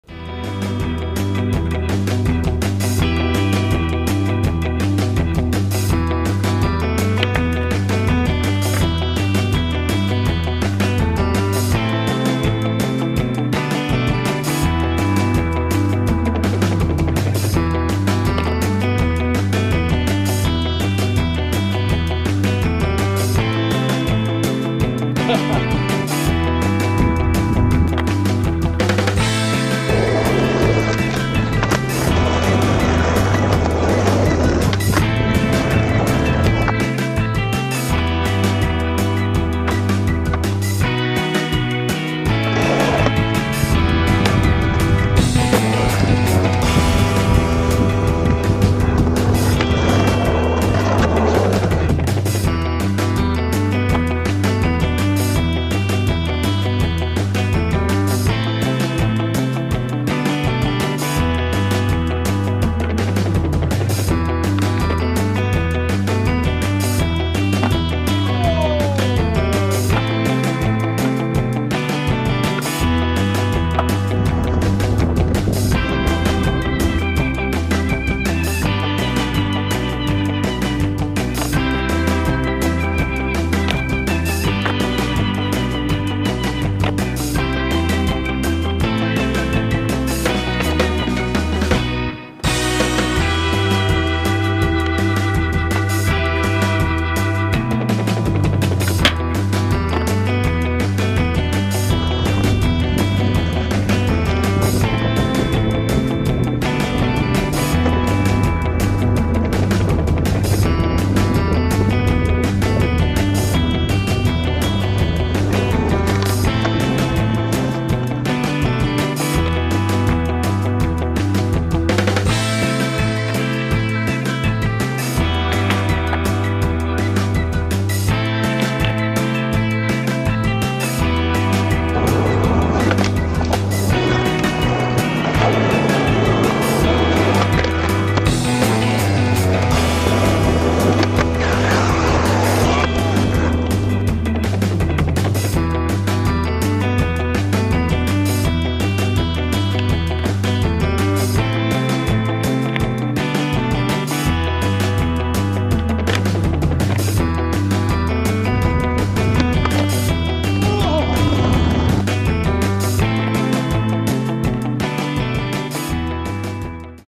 Vuosaari SkatePark (Vuokki Sessions)
Vuoden ekat ulkona Year 2022 First Outdoor Sessions With Homies Niago, Sami, Barbarian The Animal